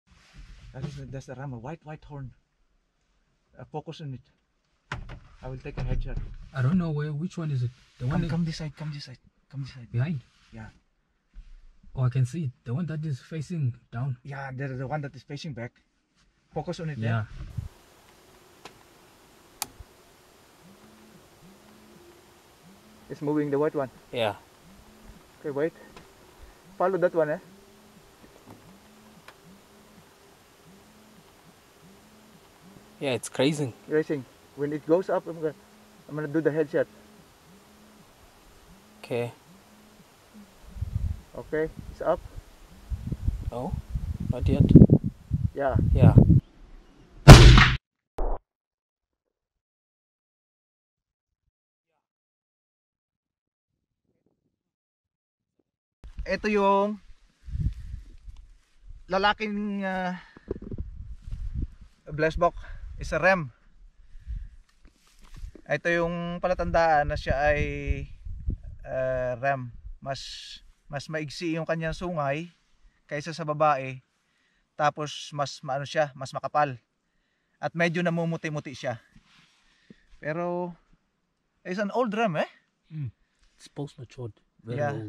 EP20|| 150M. HEADSHOT !! | HUNTING EASTERN CAPE | SOUTH AFRICA
WATCH | BLESSBOK GETS HEADSHOT FROM 150M! EASTERN CAPE SOUTH AFRICA.